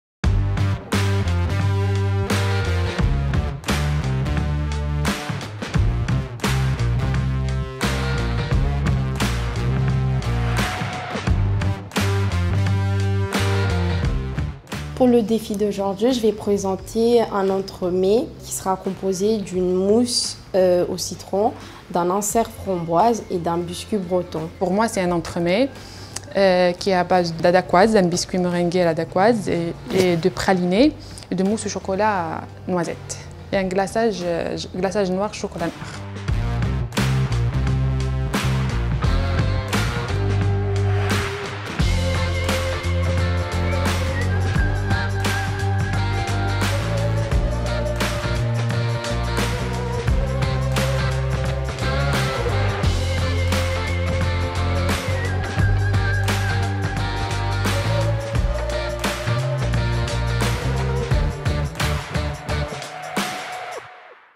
Série Graines de Pâtissier - Episode 6 - Grand défi Marseille
"A la fin du parcours les jeunes se mettent à l’épreuve et produisent chacun “une pâtisserie signature” qu’il font déguster à un jury de professionnels. Le gagnant de chaque ville se voit offrir par l’Ecole Valrhona un stage des 3 Jours avec tous les gagnants des 14 Villes : Découvrez les créations des stagiaires à Marseille cette fois-ci.

A travers cette webserie, partez aux 4 coins de la France, à la rencontre de ces jeunes en insertion professionnelle, qui suivent notre programme de préapprentissage en pâtisserie.

L'objectif ? Leur permettre de tester le métier de pâtissier et les accompagner vers leur inscription en CAP en apprentissage.

Connaissez-vous la prochaine étape dans le parcours de nos graines ? Un grand merci à tous nos partenaires !"